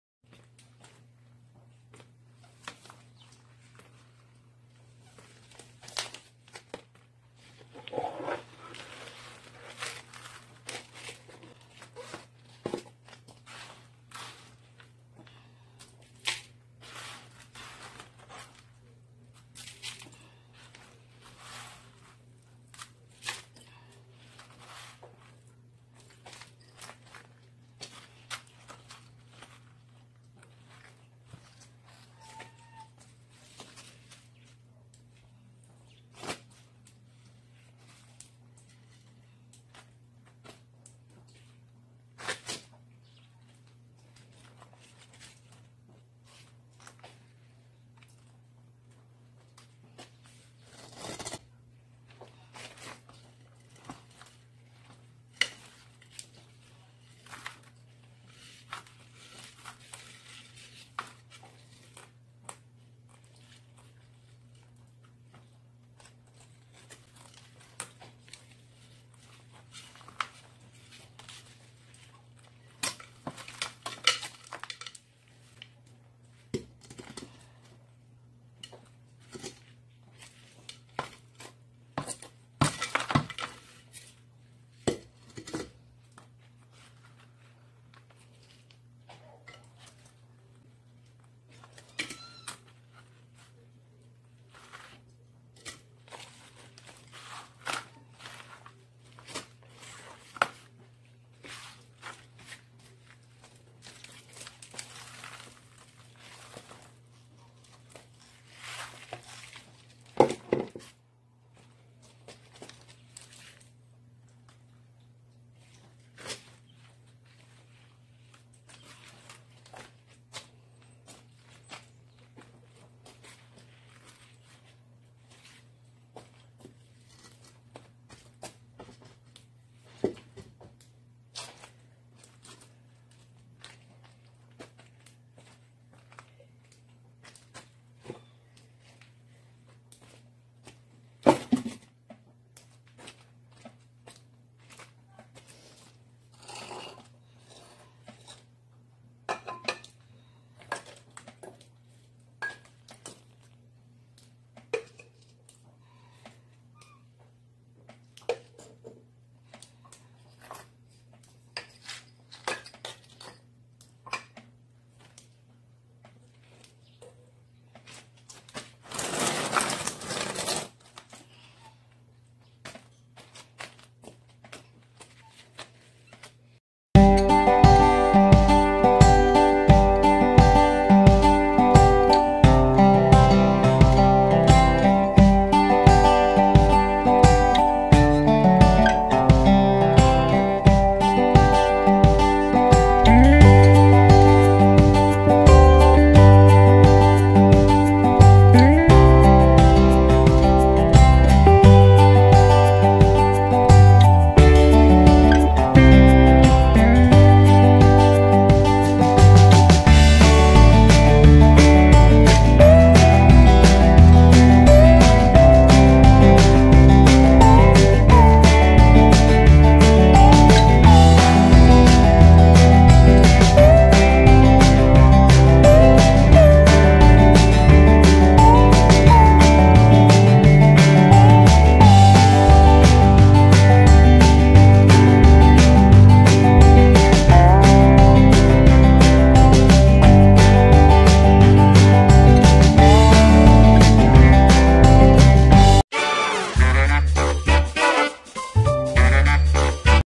금목서 분갈이 Fragrant tree repotting
봄을 재촉하는 비가 내리는 날 금목서 묘목 분갈이를 해요.
매일매일 새로운 봄꽃들도 구경오세요~^^